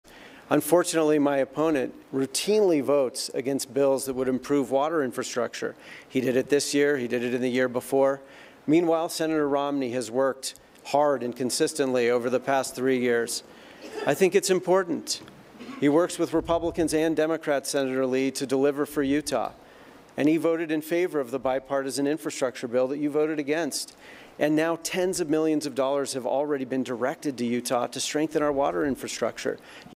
ROMNEY mention in Utah Senate debate?Yes -- by McMullin: "He works with Republicans and Democrats"
Romney mention in Utah Senate debate?
Yes -- by McMullin:
"Sen. Romney has worked hard and consistently over the past three years...He works with Republicans and Democrats, Sen. Lee, to deliver for Utah. He voted in favor of bipartisan infrastructure bill that you voted against"